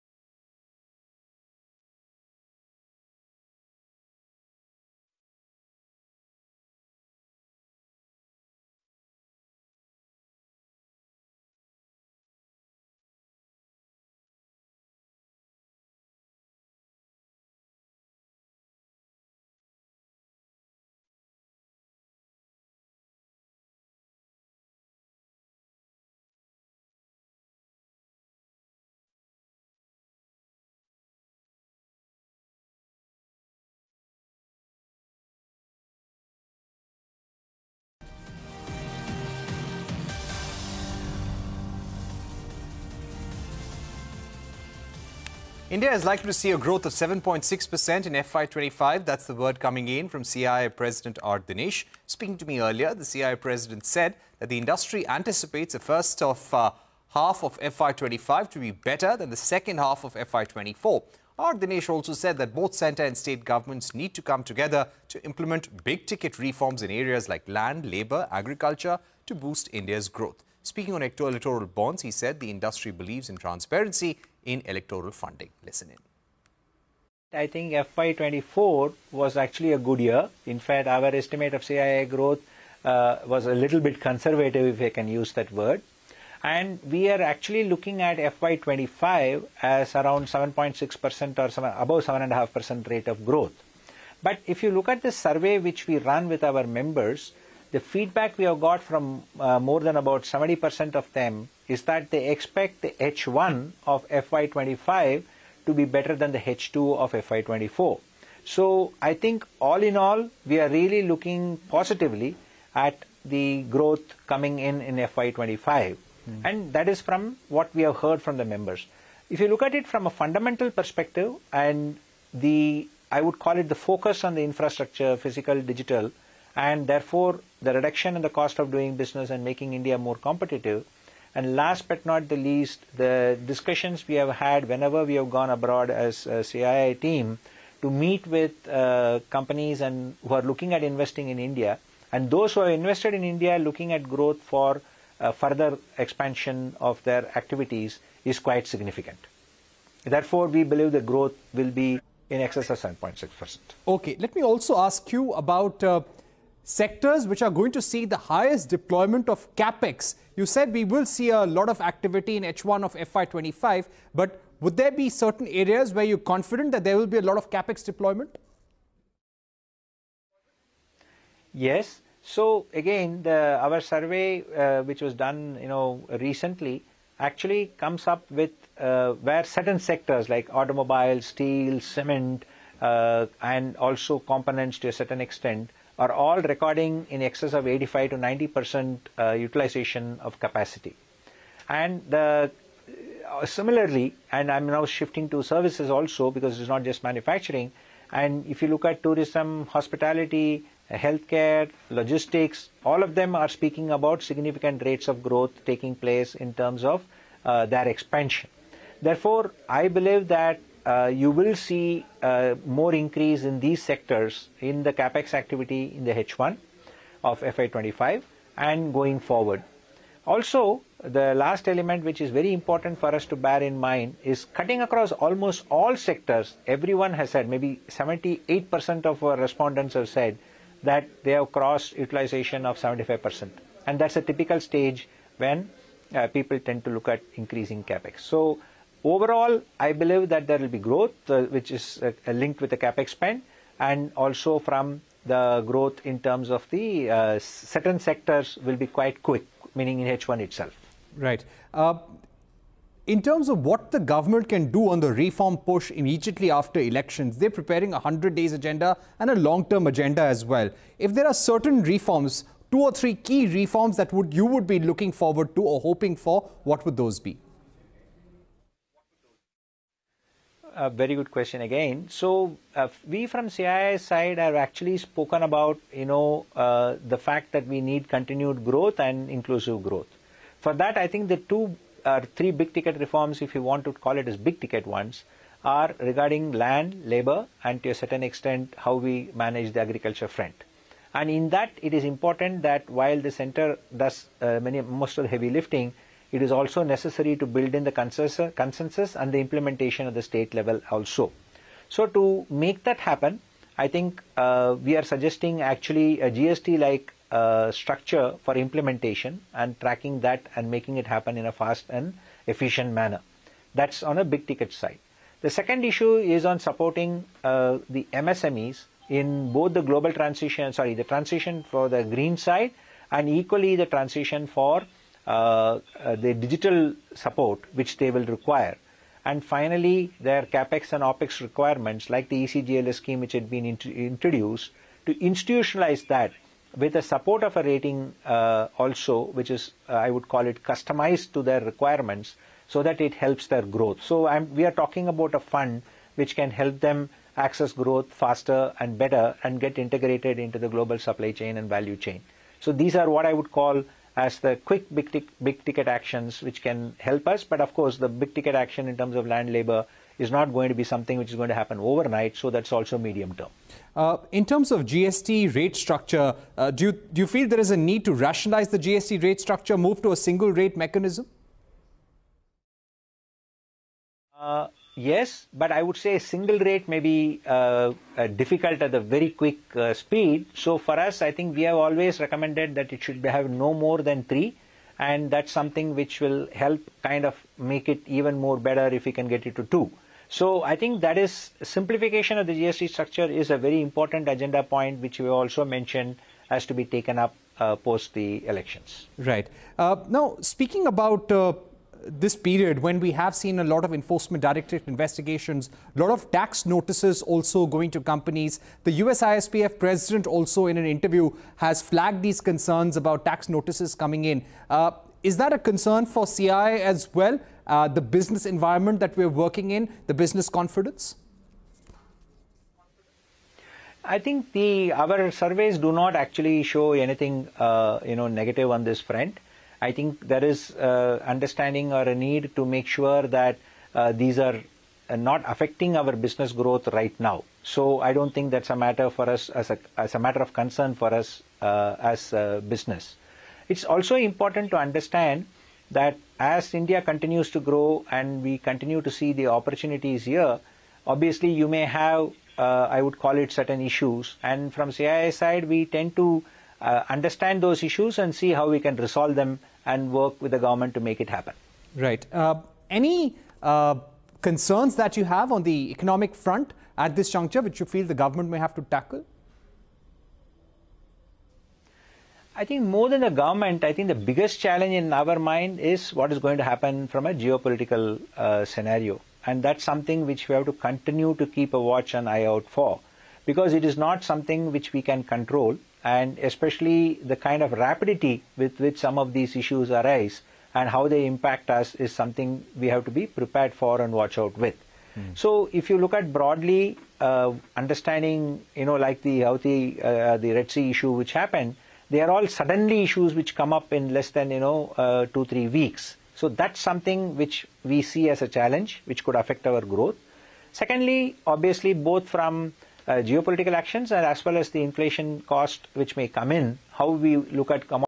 LIVE | India Is Likely To See A Growth Of 7.6% In FY25: CII President | Newscentre | CNBC TV18
India is likely to see a growth of 7.6% in FY25, according to a CII Survey. The industry body also anticipates that the first of half of FY25 to be better than the second half of FY24. @Parikshitl speaks to R Dinesh, President, CII on the CII survey and concerns around GST rationalisation & need for big-ticket reforms.

n18oc_business